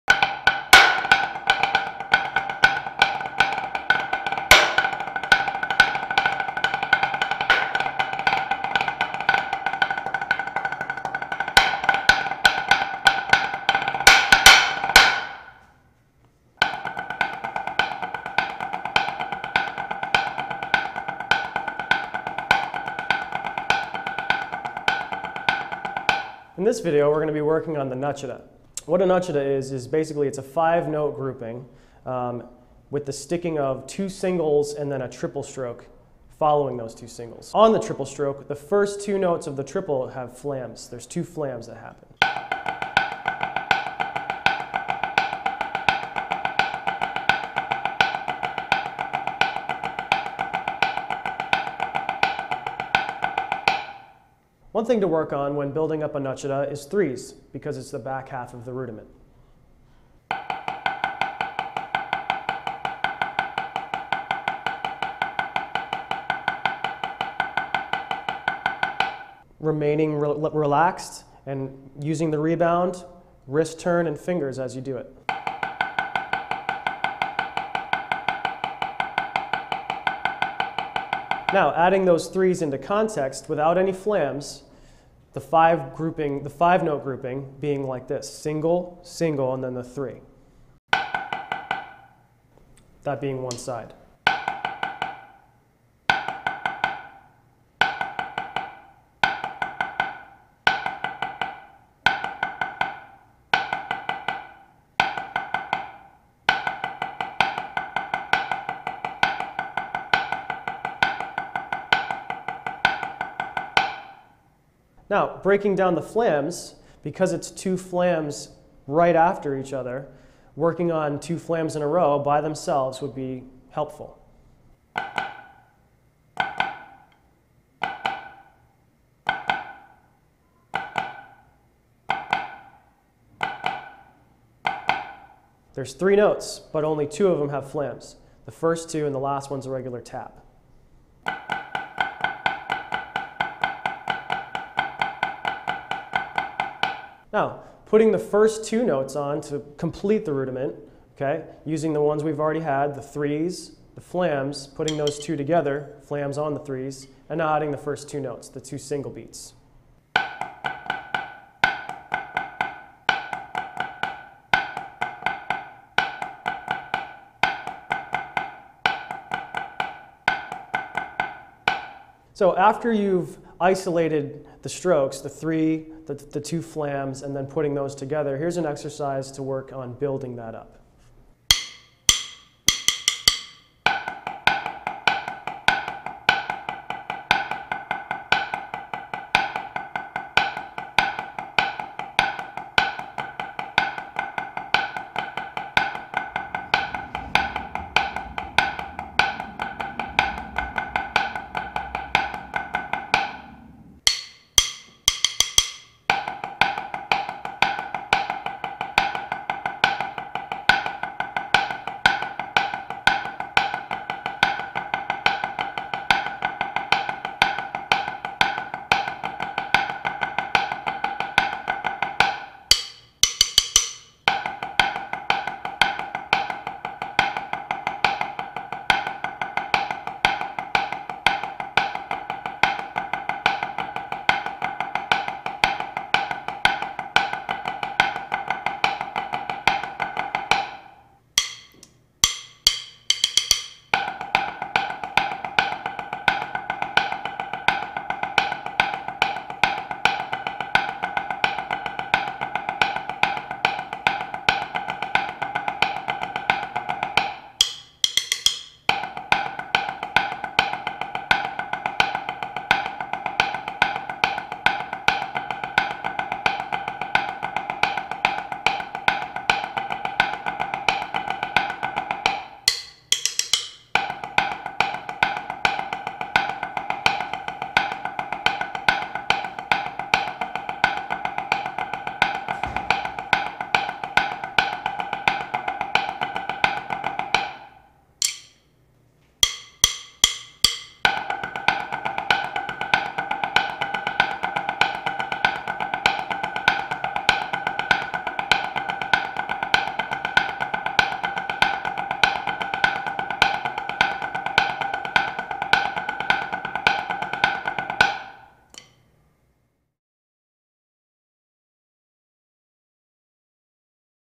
Hybrid Rudiment: Nuttchada / Vic Firth Educational Series
In this video lesson feature, Vic Firth covers a portion of the vast library of Hybrid Rudiments — broken down, explained, and demonstrated by some of the world's premier percussion instructors and performers!

to see the music as well as get exercises to help you learn and expand your hybrids!